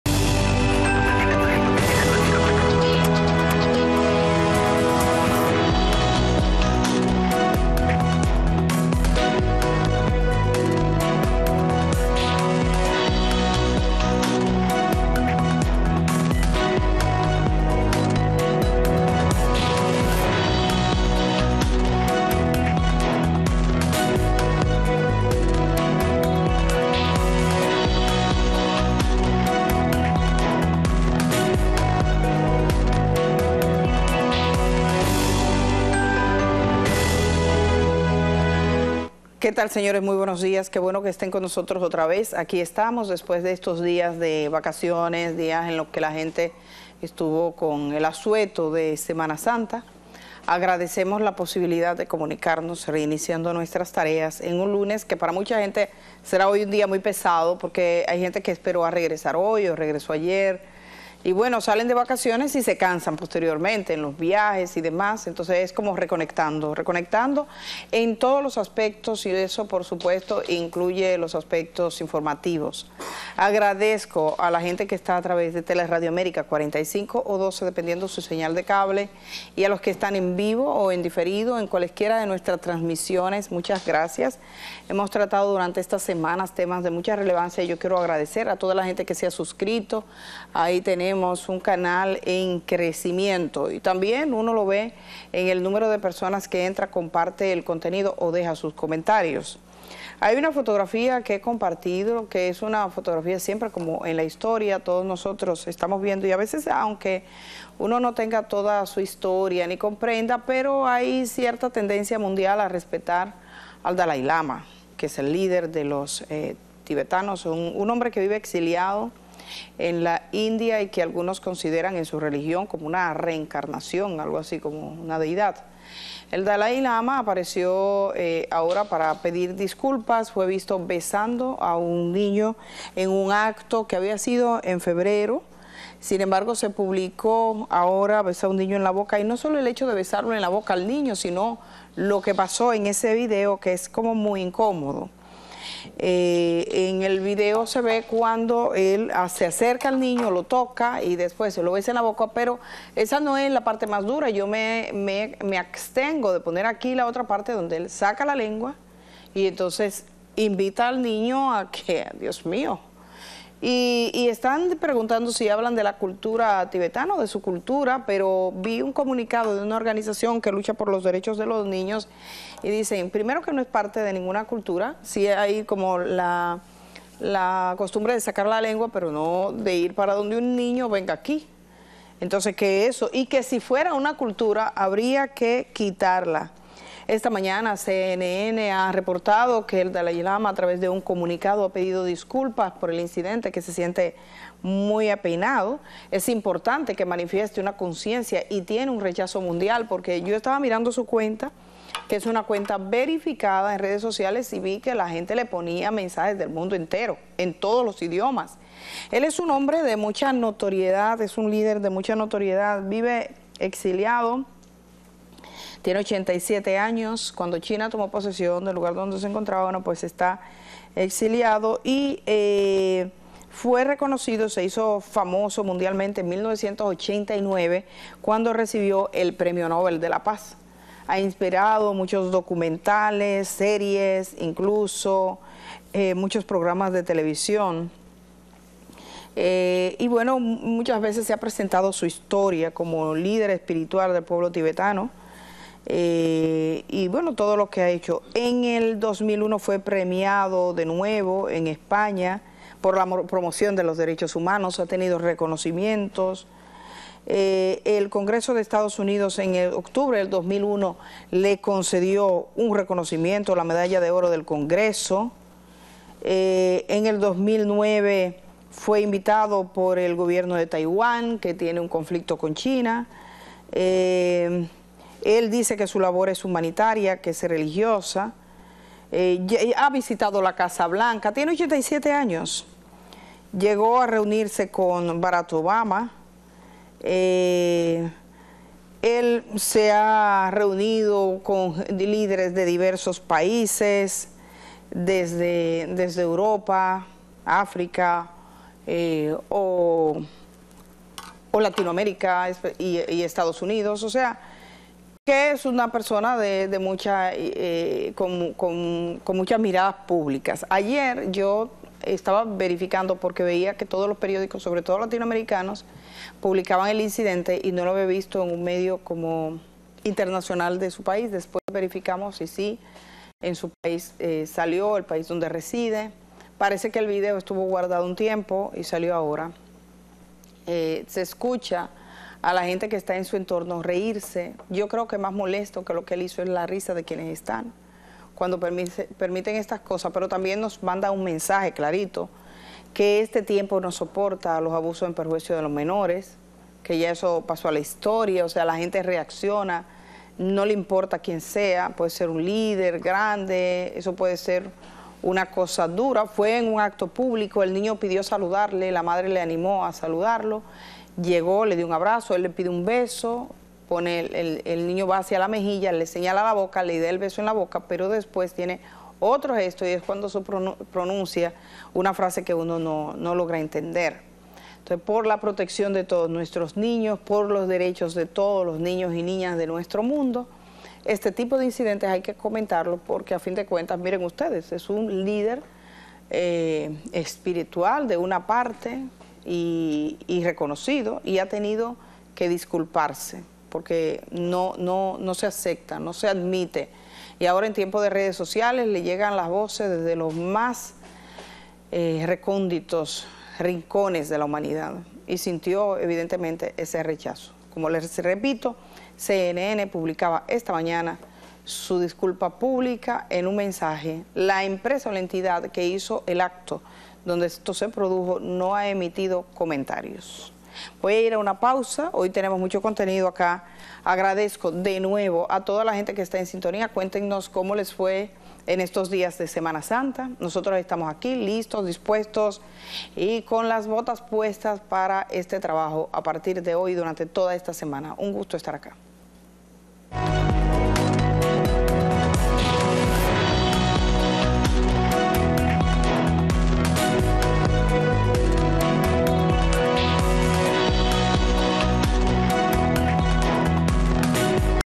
Dalái Lama pide excusas por video donde b̶e̶s̶a̶ u̶n̶ n̶i̶ñ̶o̶ en la India el pasado mes de Febrero
Canal Oficial Periodista Edith Febles